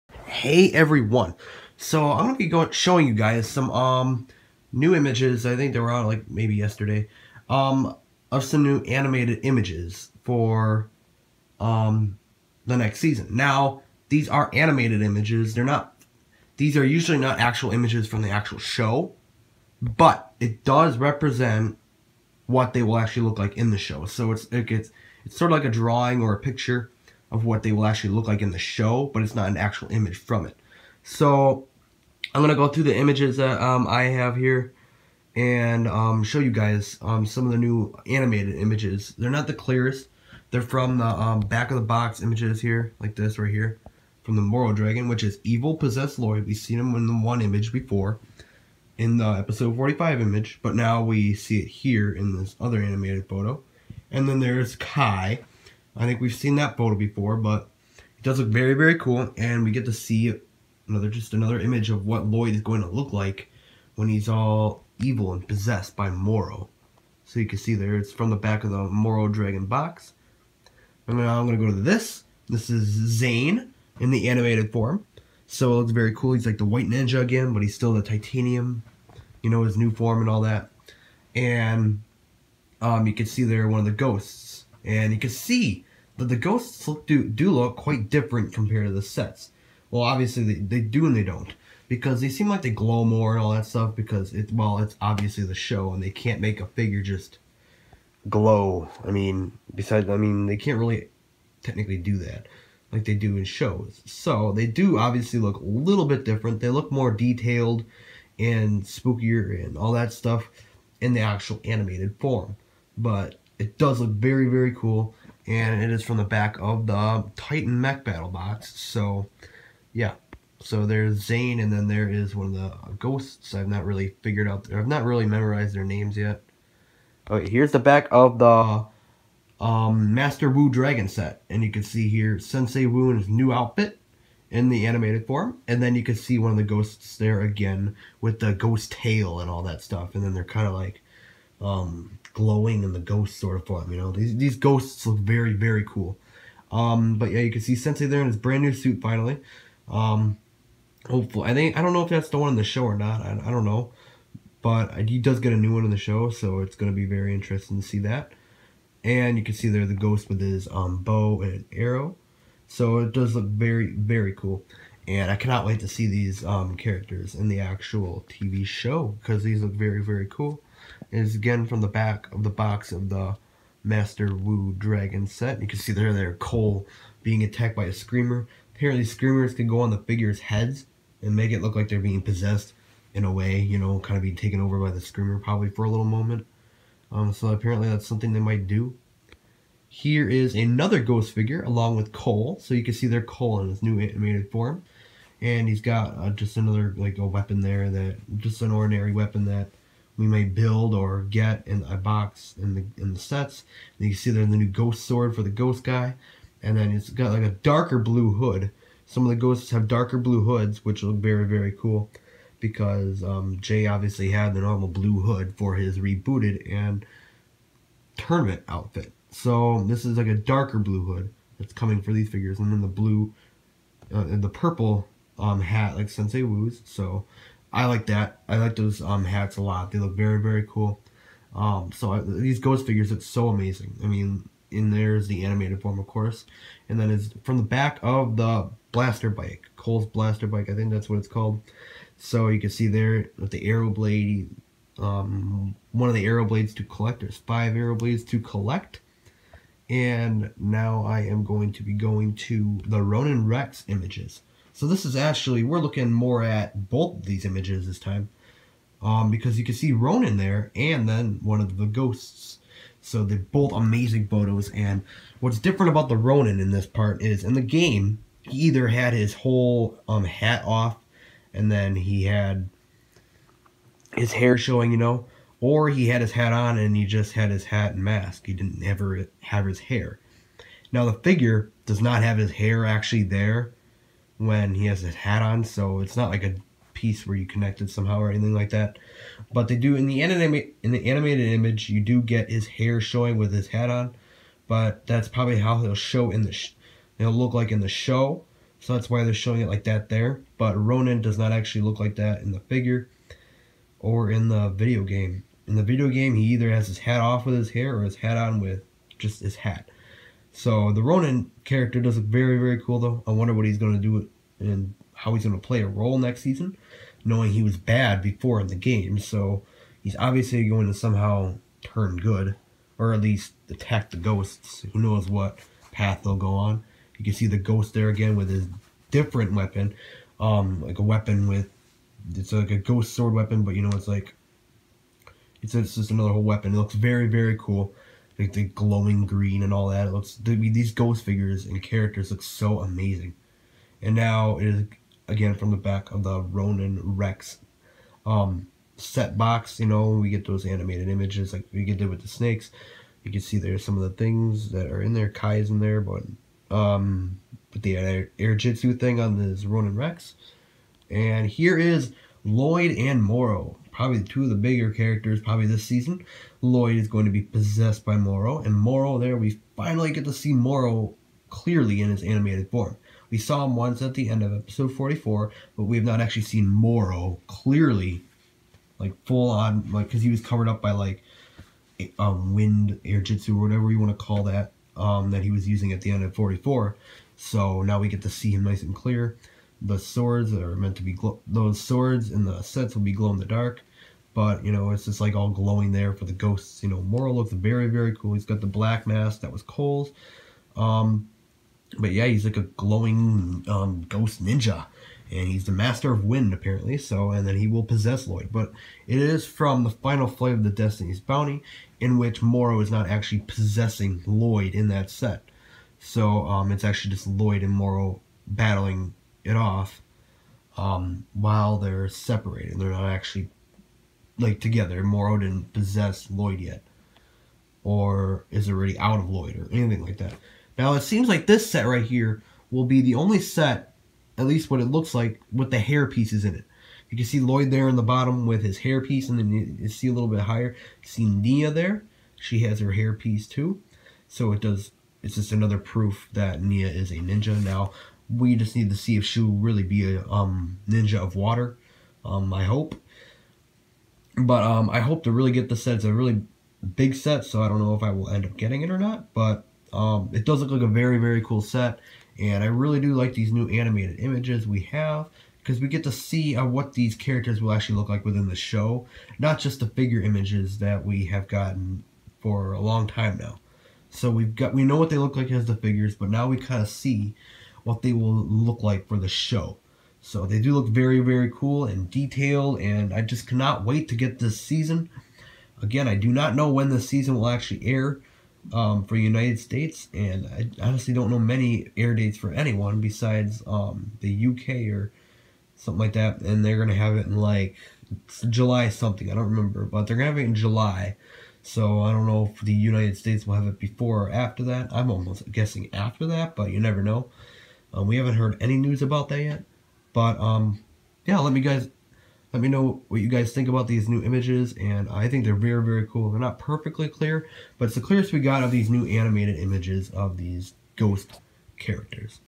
New Ninjago Summer 2015 Animated Character Images!
I'm here to show you guys a run though of the images from the backs of the set boxes that show new animated images of characters coming in the Ninjago Summer 2015 Season 5.  What do you think about these?